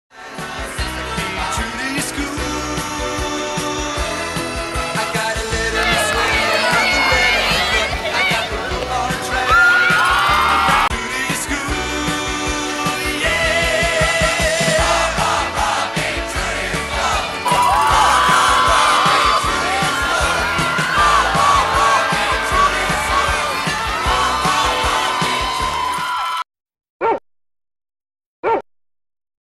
Mt Ararat and Morse Cheerleaders in an acrobatic Smack Down
The Eagles and Shipbuilders engaged in a spontaneous, unscripted smack down at half time of the Mt Ararat Morse football game featuring multiple somersaults and flips. Wow!